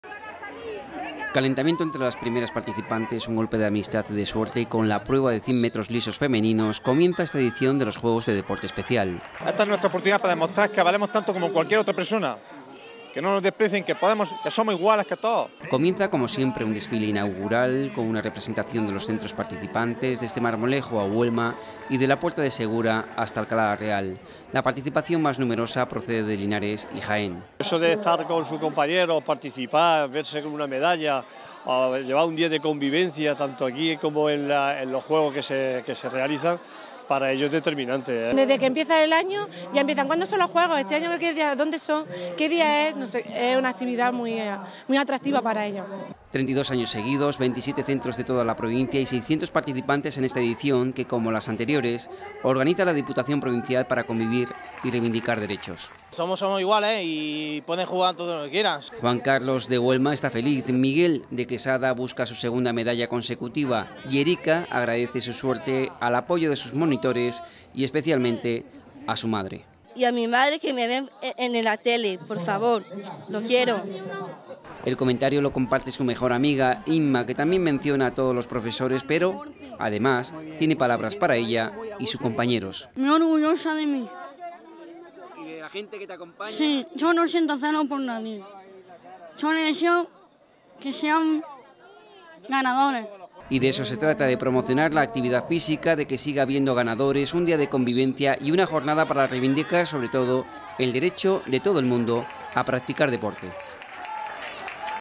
Más de 600 participantes en los XXXII Juegos Provinciales de Deporte Especial en Baeza
Más de 600 participantes de 27 centros de la provincia de Jaén participan en los XXXII Juegos Provinciales de Deporte Especial en Baeza.